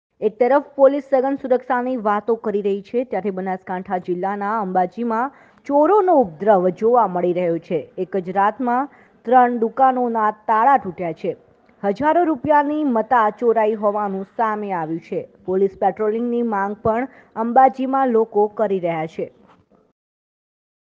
ambaji એક જ રાતમાં ત્રણ દુકાનોના તાળા તૂટ્યા....
અંબાજી બ્રેકિંગ.
અંબાજીમાં વધતો ચોરો ઉપદ્રવ. એક જ રાતમાં ત્રણ દુકાનોના તાળા તૂટ્યા.
હજારો રૂપિયાની મતા ચોરાઈ.
પોલીસ પેટ્રોલિંગની માંગ .